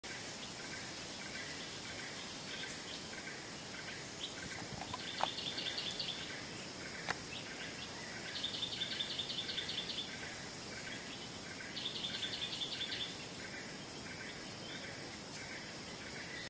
Something strange
Found on a tree in Prey Long Cambodia. No idea what it is.